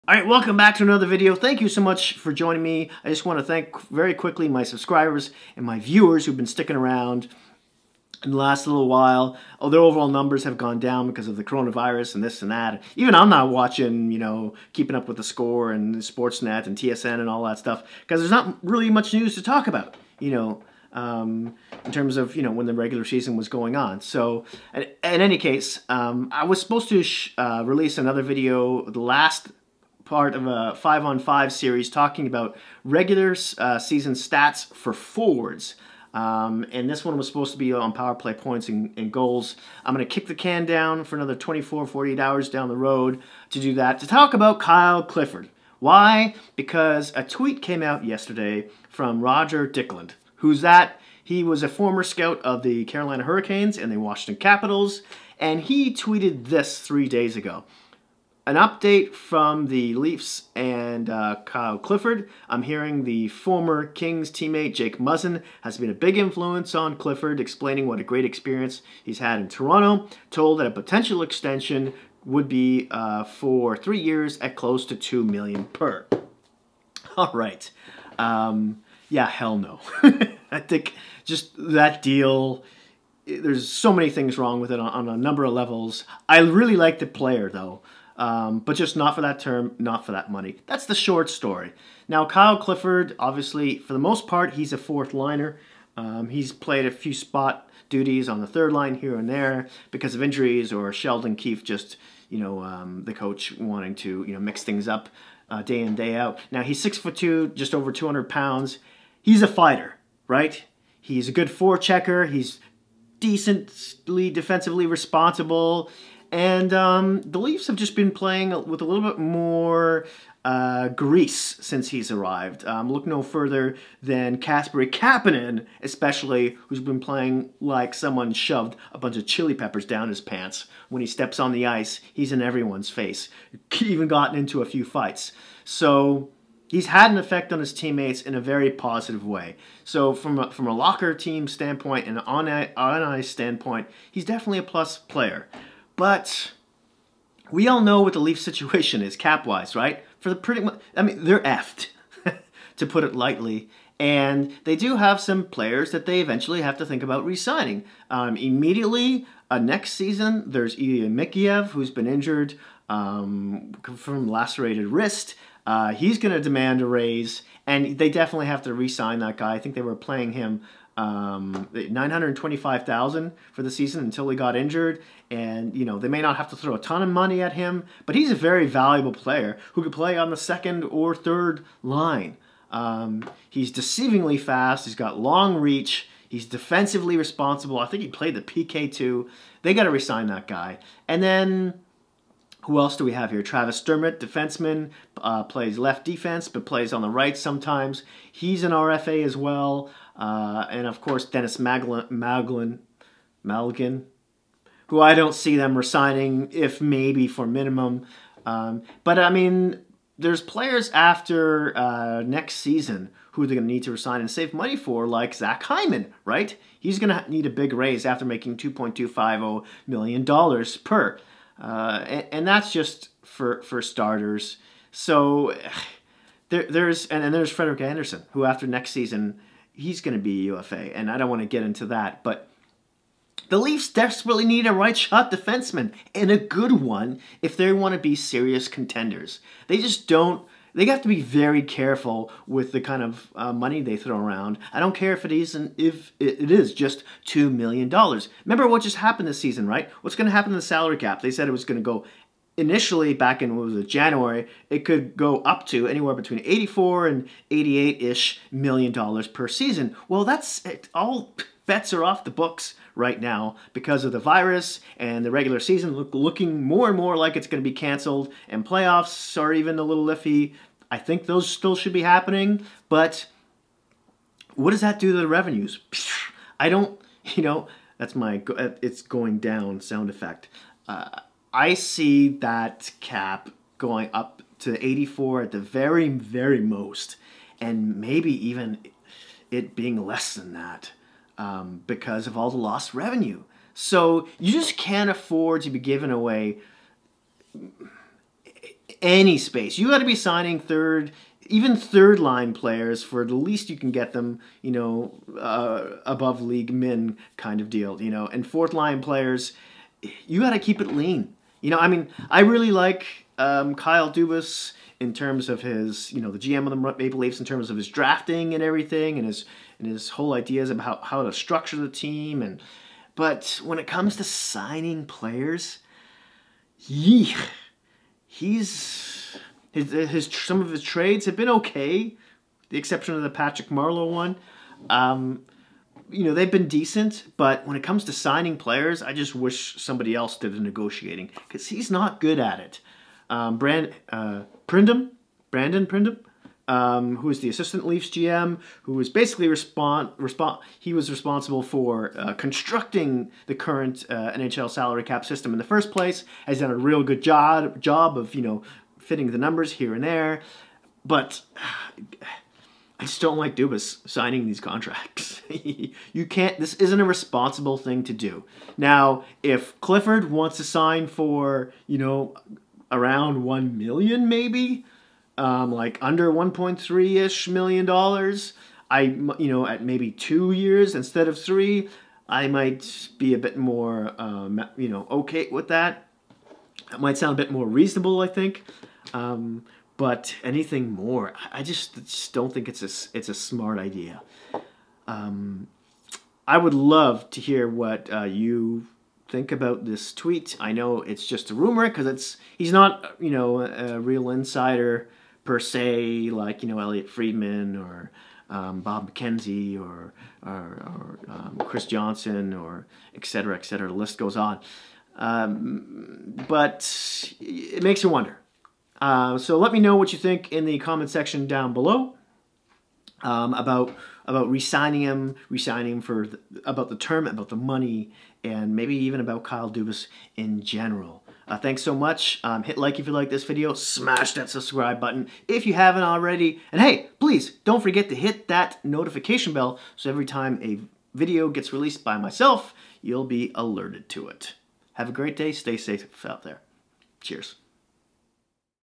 Rumour of Leafs re-signing Kyle Clifford
Whether you like Kyle Clifford or not,  a rumoured 2 million at three years is not a responsible signing for the Toronto Maple Leafs at this point. Not to mention I forgot to mention in the video that the Leafs will have to cough-up a 2nd round pick instead of a 3rd if he re-signs with them. Lets hear your comments down below!